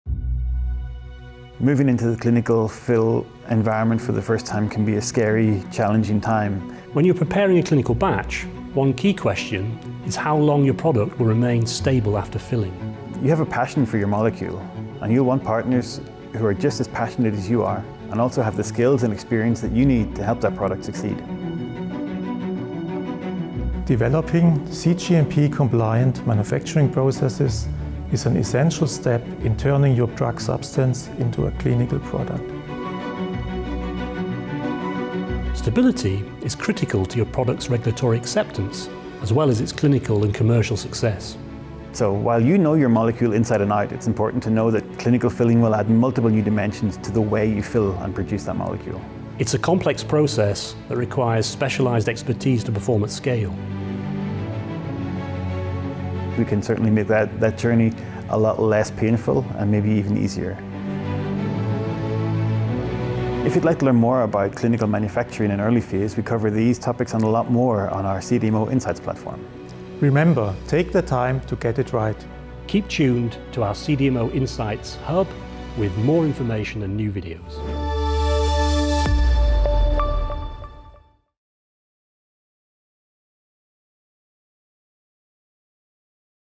New video series on planning a successful clinical batch | CDMO Insights
The pivot from preclinical development to in-human trials is one of the most important transitions in the early life cycle of an injectable medication. A key step: Producing a batch of clinical material to support your in-human trials. In this series, our experts will explain how your team can plan an efficient, streamlined clinical batch project that sets you up for success in your trials and beyond.